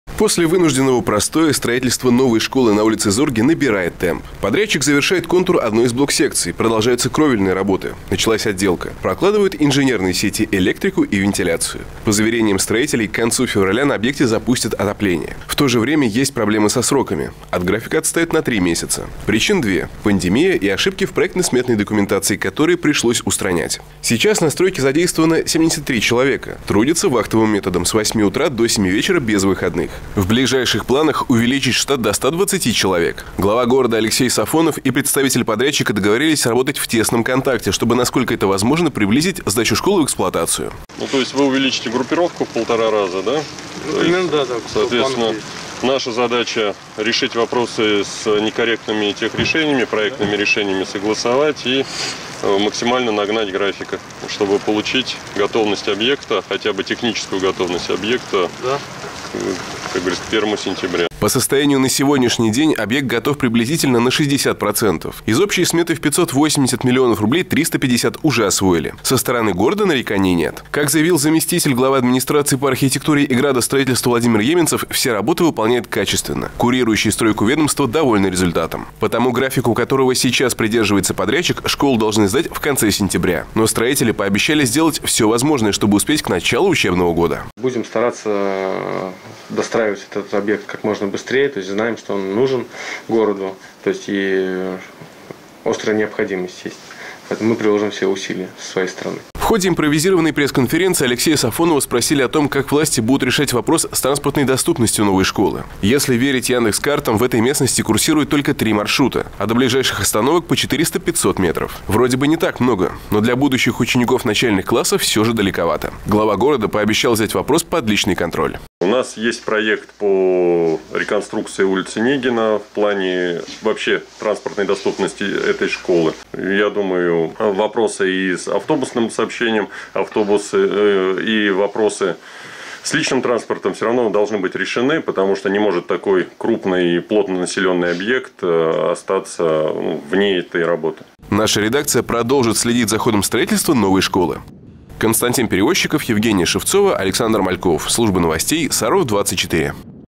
Когда откроют новую школу на ул. Зорге?
Из-за пандемии коронавируса и ошибок в проектно-сметной документации строительство новой школы на улице Зорге идёт с отставанием графика на 3 месяца. Здание готово на 60%.